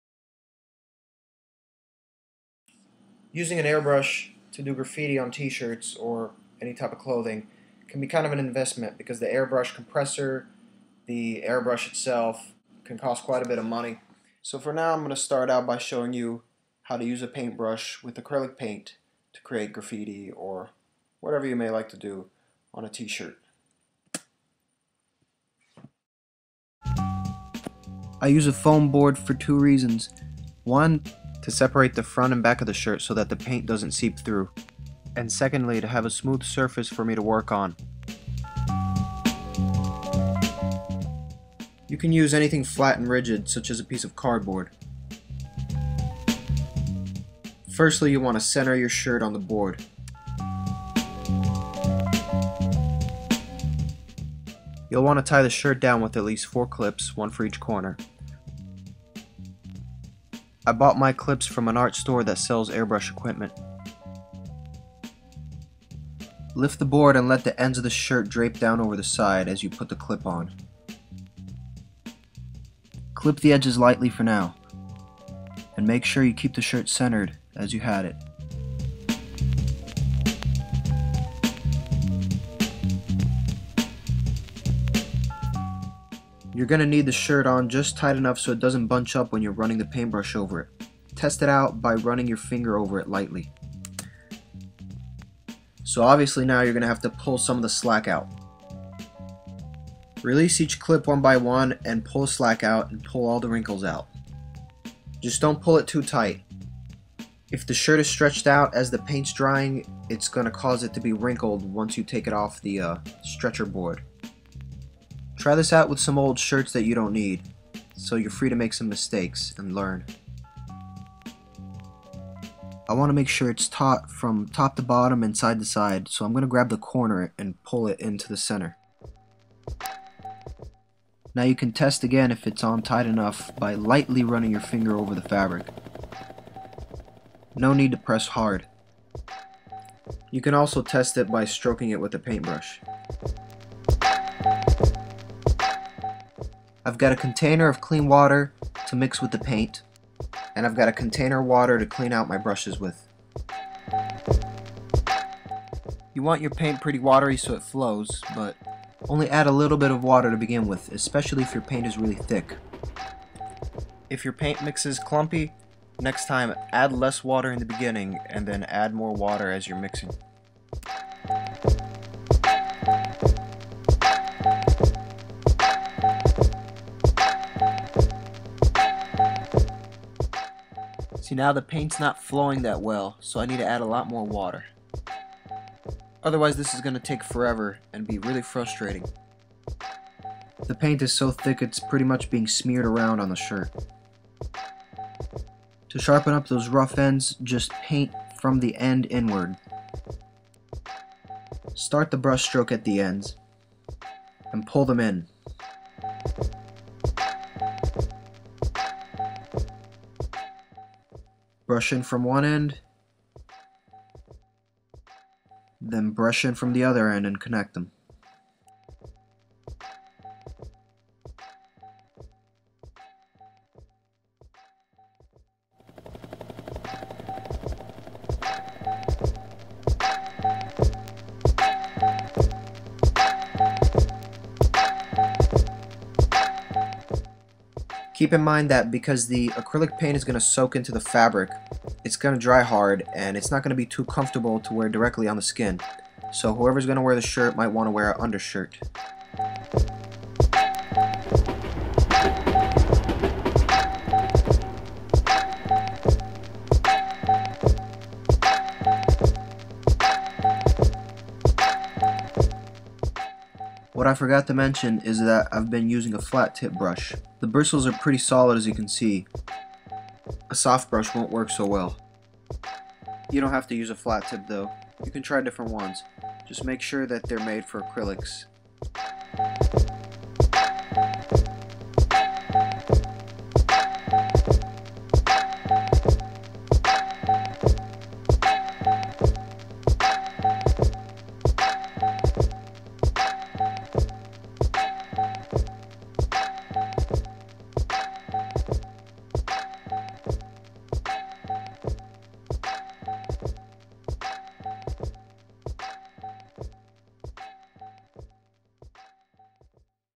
Reskew's Graffiti Tutorial #4 Acrylic painting on Clothing MWNYC
Some suggestions on how to paint on clothing with acrylic paints, without than using an airbrush.